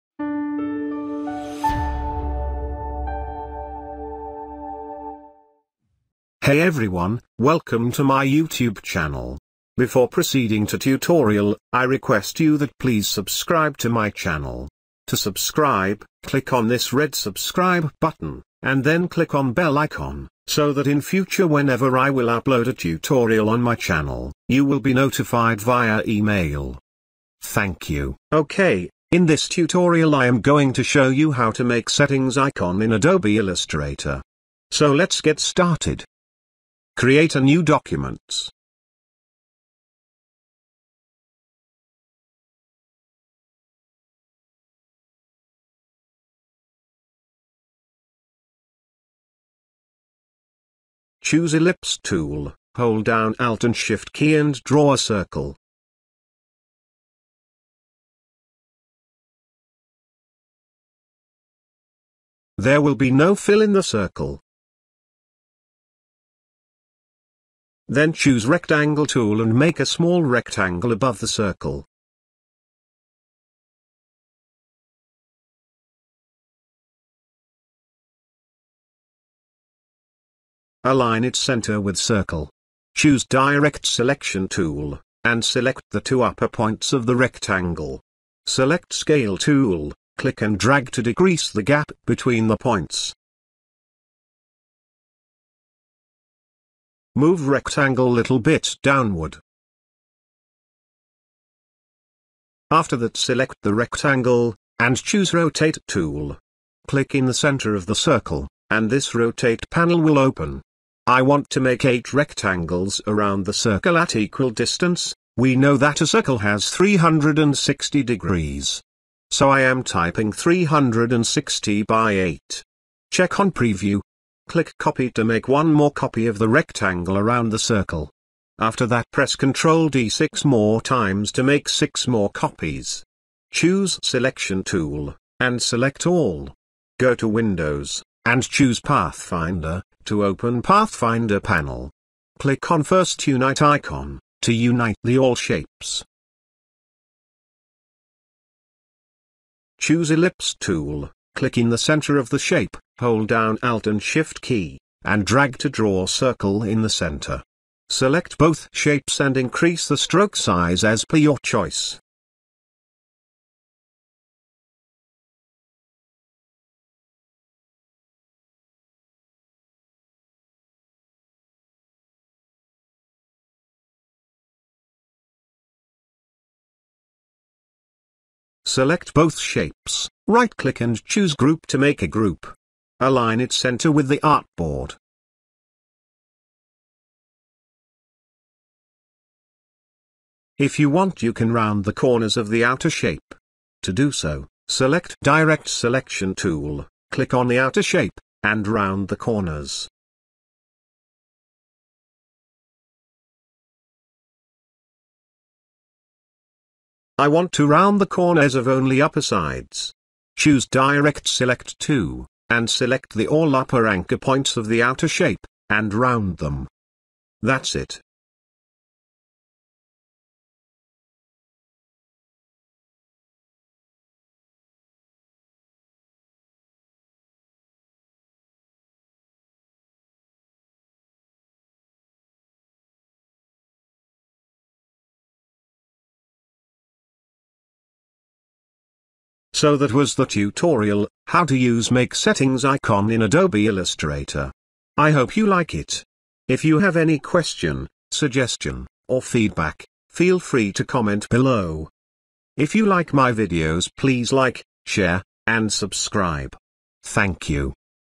How to make Settings Icon in Adobe Illustrator
How to make settings icon in adobe illustrator. Welcome to another Adobe Illustrator Tutorial of Graphic Design Tutorials series. In this tutorial we are going to make settings icon.

To make a settings Icon we will use ellipse tool, rectangle too, rotate tool, and unite option inside pathfinder panel.

I hope you like this adobe illustrator tutorial about how to make settings icon in adobe illustrator.